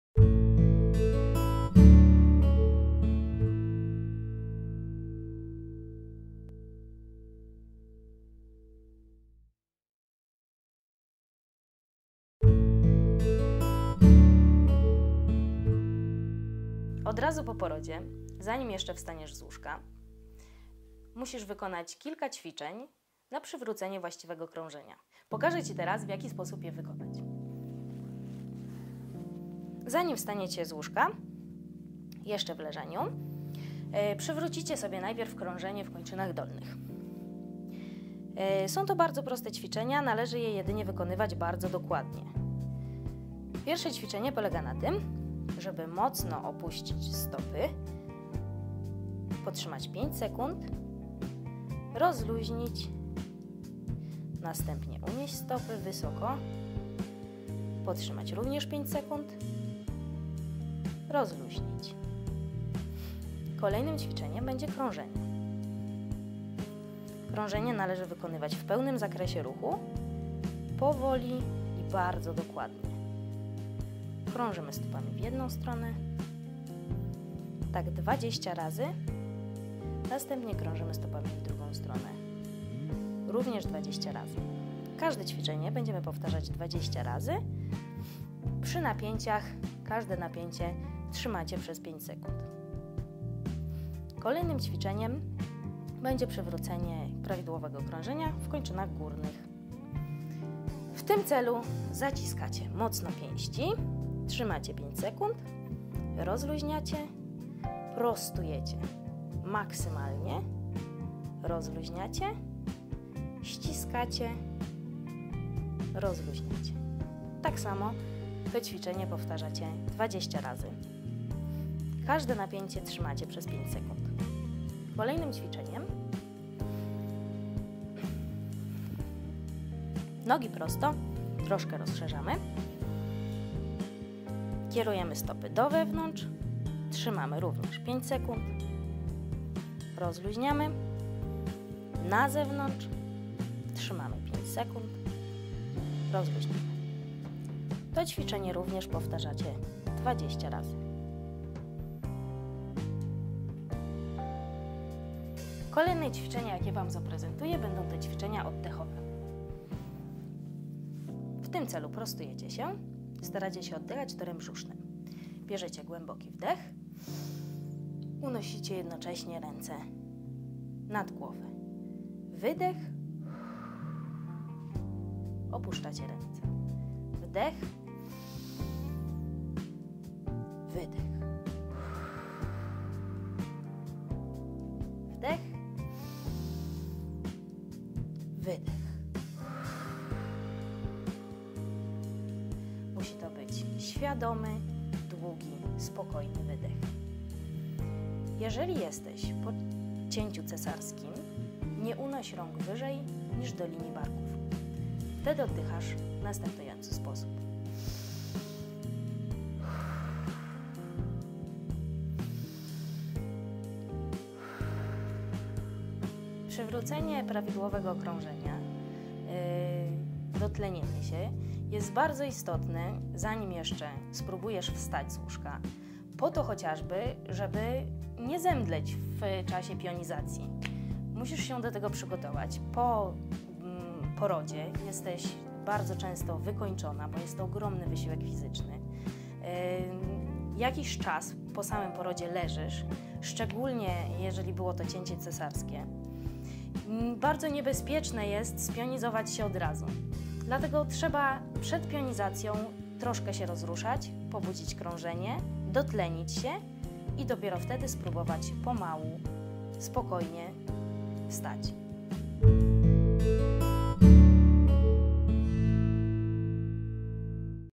Jak się uruchomić po porodzie?
Jak wstać z łóżka po porodzie? Powoli i najlepiej po wykonaniu kilku ćwiczeń krążeniowych i oddechowych.
Leżąc na plecach wykonaj 20 powtórzeń każdego z przedstawianych ćwiczeń. Każde napięcie utrzymuj min. 5 sekund.
1. stopy (w górę, rozluźnij, w dół, rozluźnij i krążenie stopami)
2. dłonie (zacisnąć pięść, rozluźnij itd)
3. wyprostuj nogi, lekko rozsuń, pięty leżą na podłożu (stopy do wewnątrz, rozluźnić, na zewnątrz, rozluźnić)
Kolejna partia to ćwiczenia oddechowe. Siedząc na łóżku weź kilka głębokich oddechów. Podczas wdechu unoś ręce do góry (inaczej po cięciu cesarskim-zobacz), z wydechem pomału opuszczaj.
Te ćwiczenia pomagają rozruszać się i dotlenić przed wstawaniem aby zapobiec np. omdleniu.
Powodzenia;)!
Kolejne materiały z Poradnika poporodowego z fizjoterapeutą Agnieszką Chmielnicką niebawem na naszym kanale YT i
Zapraszamy!